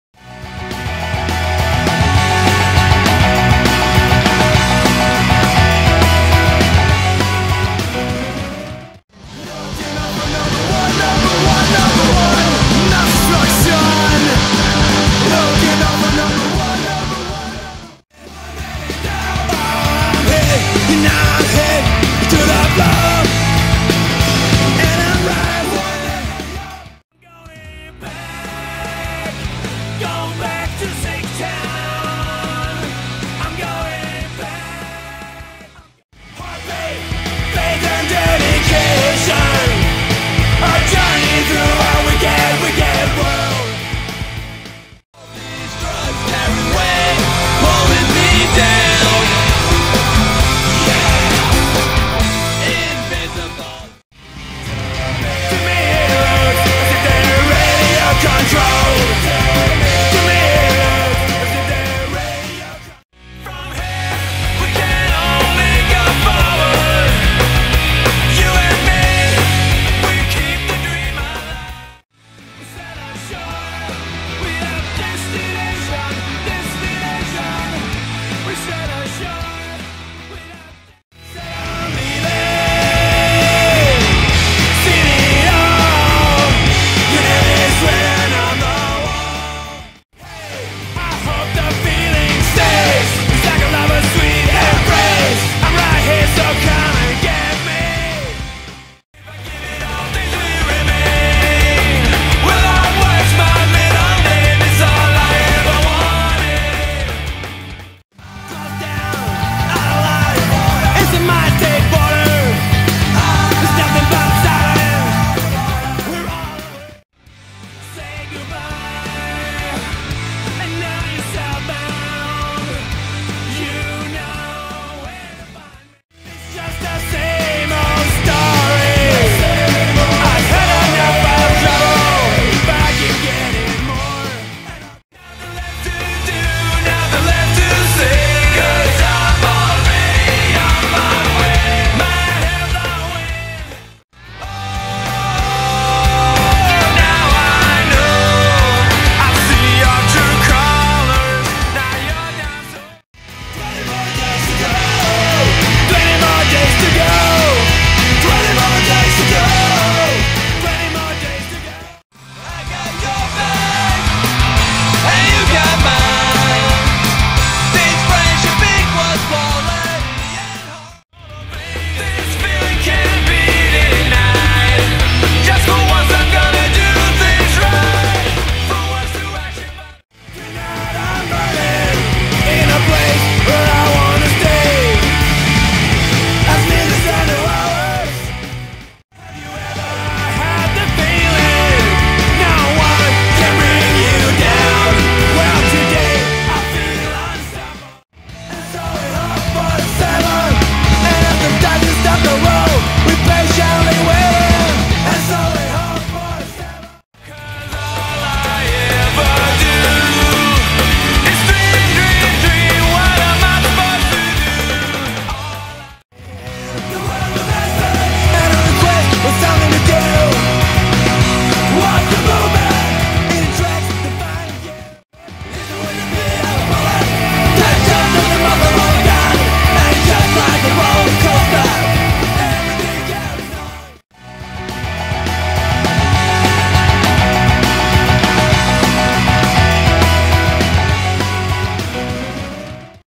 BOMBSHELL ROCKS - Top 25
Bombshell Rocks is a Swedish band that was founded in 1995, in Västerås, Sweden.
Punk Rock
Street Punk